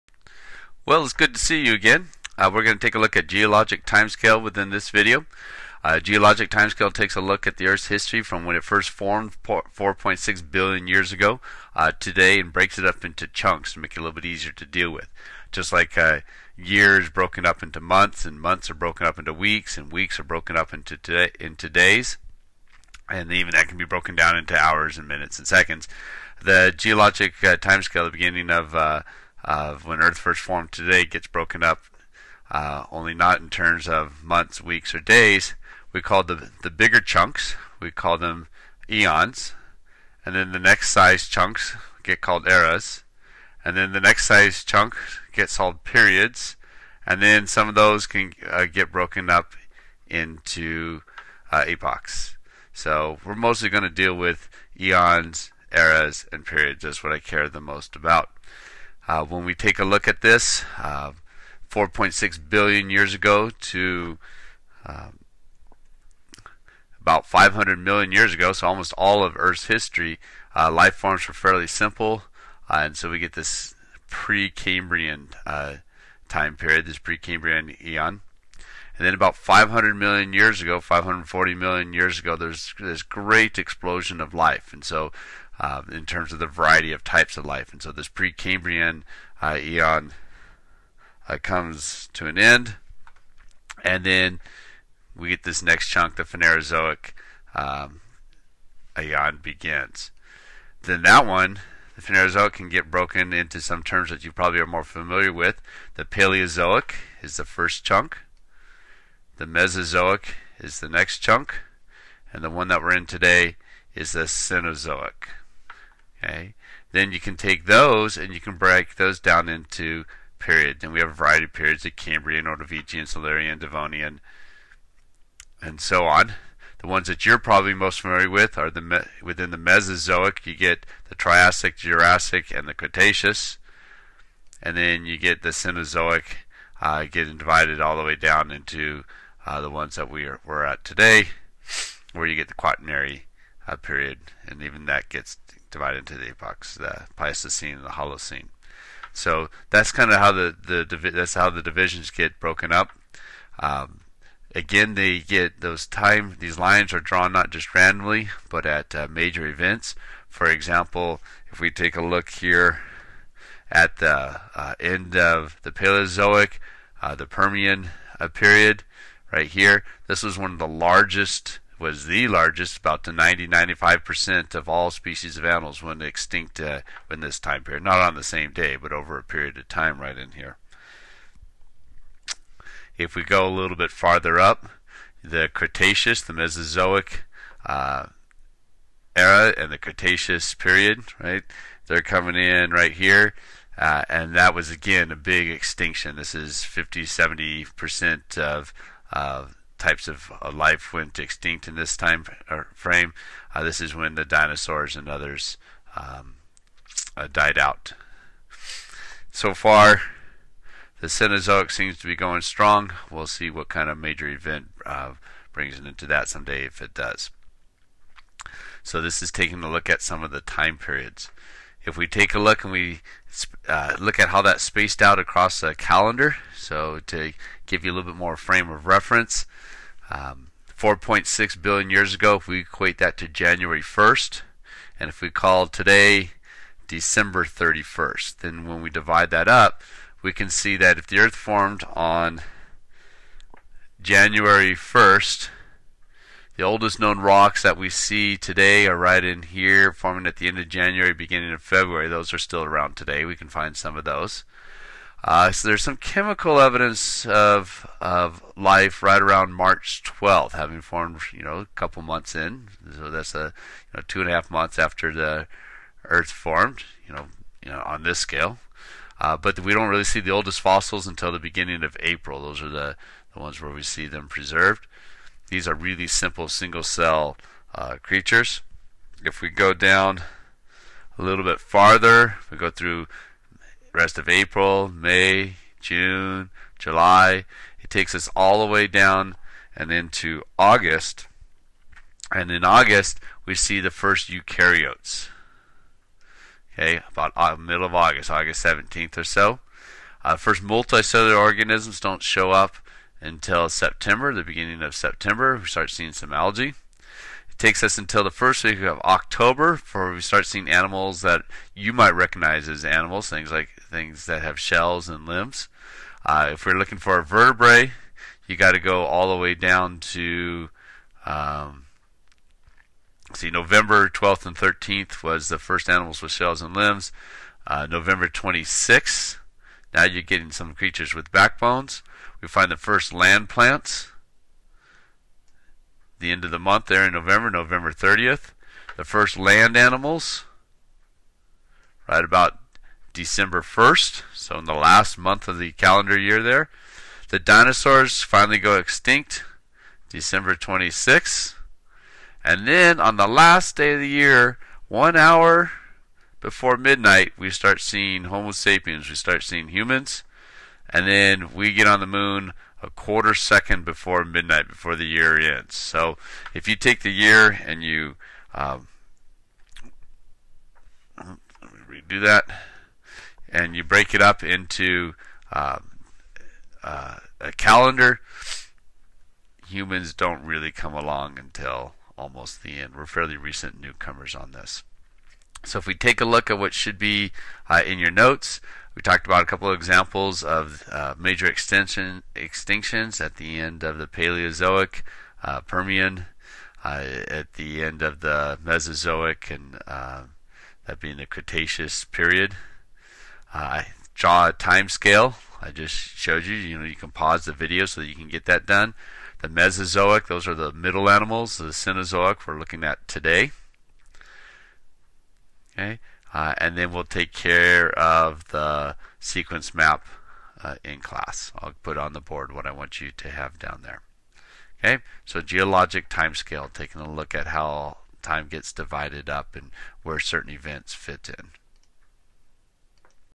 Geologic Time Scale.mp4
Brief description of divisions within geologic time scale

Table of Contents:

00:00 - Geologic Time Scale
03:00 - Example of Major event
03:43 - Earth History as One Calendar Year
06:26 - Review